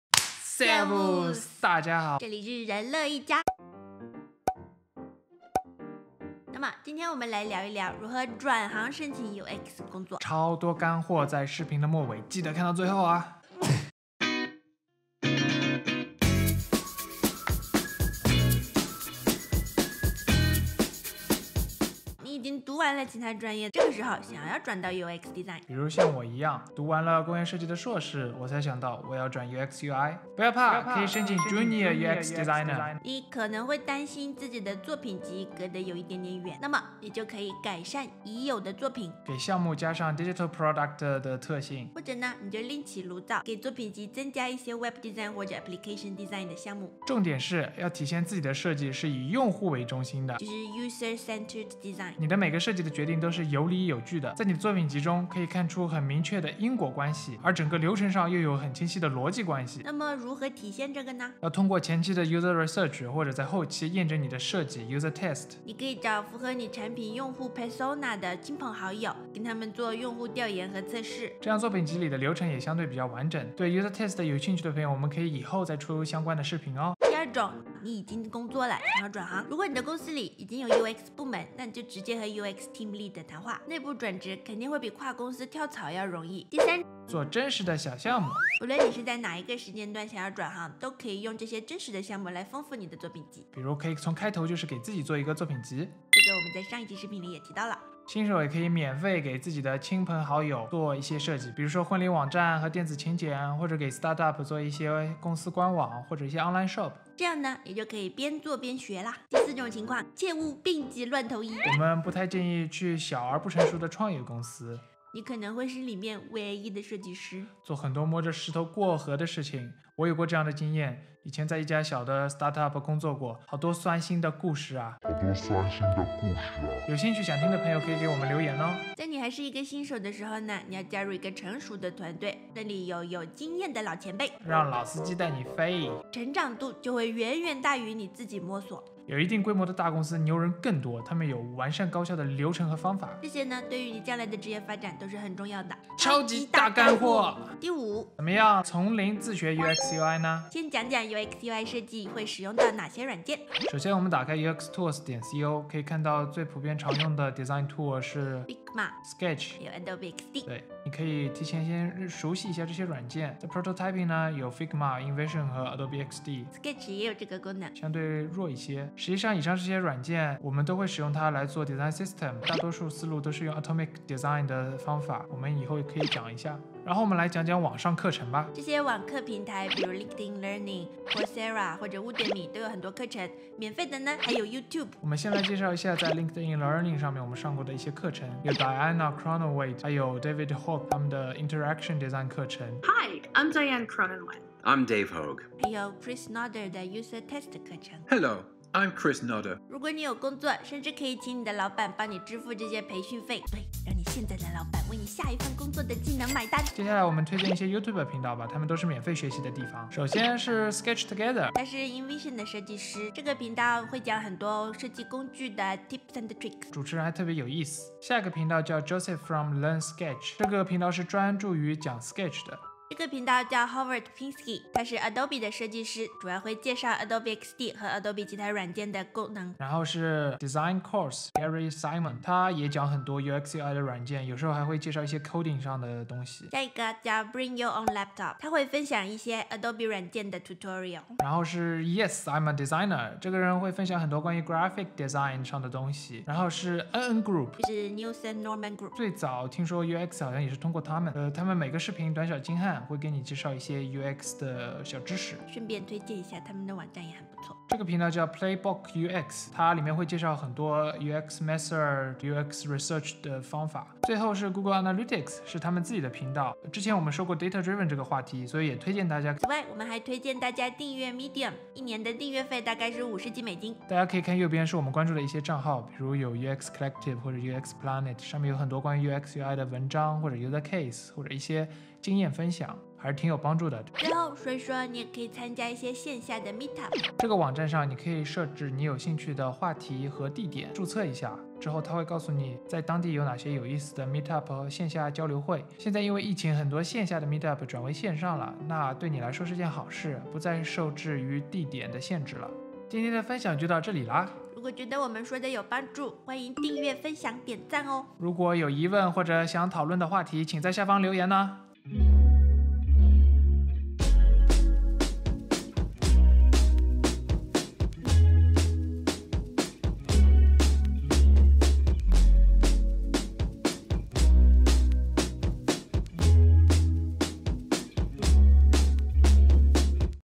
超推薦的8個YouTube頻道和3個LinkedIn線上課程，自學轉行 UX 用戶體驗設計師的Tips！ | How to change career to UX/UI Designer
如何轉行？如何練習？應該自學什麼？在哪裡學？幾個觀眾留言問我們是如何從工業設計/產品設計，轉專業換到 UX/UI Designer 用戶體驗設計師（交互設計師、網頁設計師、介面設計師）的工作。這個視頻裡分享的所有經驗和建議都來自於我們自己以及身邊朋友親身經歷，我們認識的人從產品設計、平面設計和包裝設計，還有心理學、語言學，零經驗轉職到UX設計師的都有喔！希望對你有幫助。

影片章節：
0:00 話題簡介，如何轉行申請 UX 設計師的工作
0:21 剛剛畢業，立即優化作品集轉到 UX Design
1:16 已經在工作的人，如何轉行到 UX Design？
1:26 有哪些真實的網頁設計小項目可以用來練習以及豐富作品集？
1:50 不要為了轉行而接受糟糕的工作！
2:26 超級大乾貨：如何從零自學 UX/UI Design
2:32 熟悉一下 UX/UI 網頁設計和APP設計常用的軟體
2:57 我們推薦的 UX UI 網頁設計 交互設計 LinkedIn Learning 課程
3:27 免費學習，乾貨滿滿的設計類 YouTube 頻道
4:30 訂閱和閱讀 Medium
4:44 參加設計師聚會 Meetup
5:03 訂閱我們吧